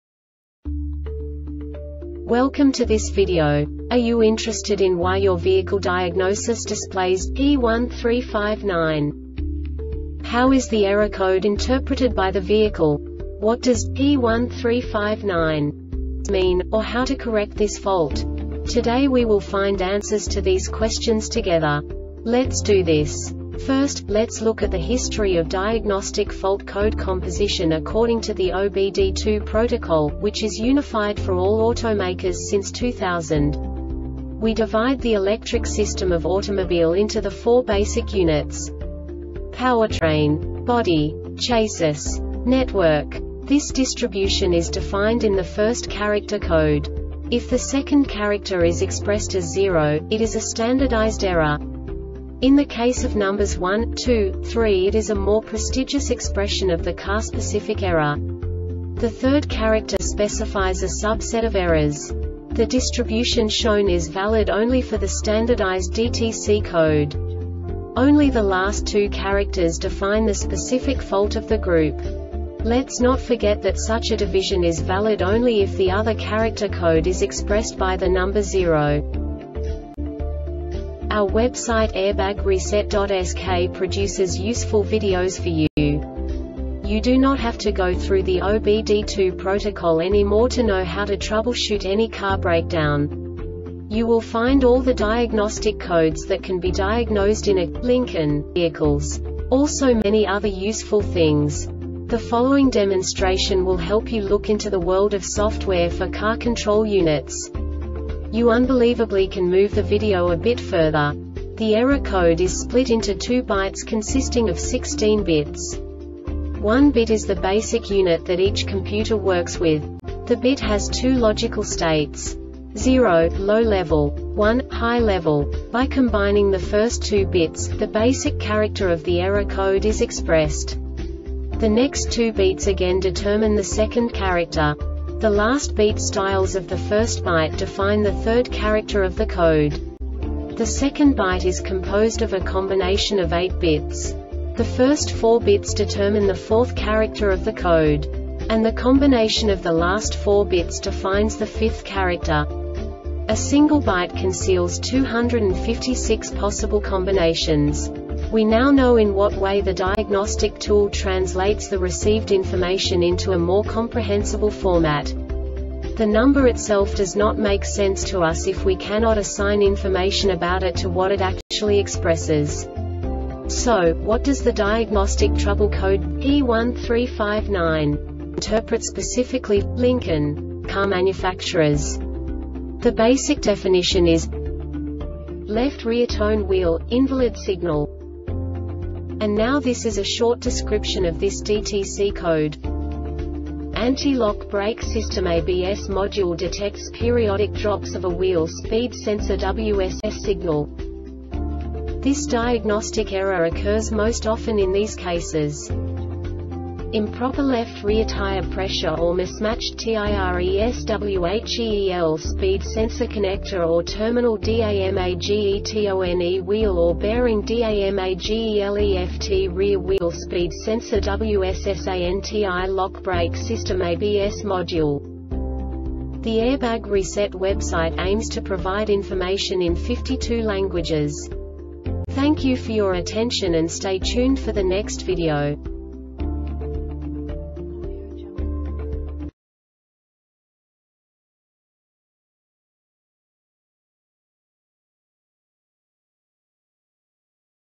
DTC Lincoln P1359 Short Explanation
Contents:
0:21 Basic DTC analysis according to OBD2 protocol standard.
1:48 Insight into programming
2:58 A diagnostically understandable description of the Diagnostic Trouble Code
3:05 Basic error definition

Make:
Lincoln
Code:
P1359
Definition:
Left Rear Tone Wheel - Invalid Signal
Description:
Anti-lock Brake System (ABS) Module detects periodic drops of a Wheel Speed Sensor (WSS) signal.
Cause:
IMPROPER LEFT REAR TIRE PRESSURE or MISMATCHED TIRESWHEEL SPEED SENSOR CONNECTOR or TERMINAL DAMAGETONE WHEEL orBEARING DAMAGELEFT REAR WHEEL SPEED SENSOR (WSS)ANTI-LOCK BRAKE SYSTEM (ABS) MODULE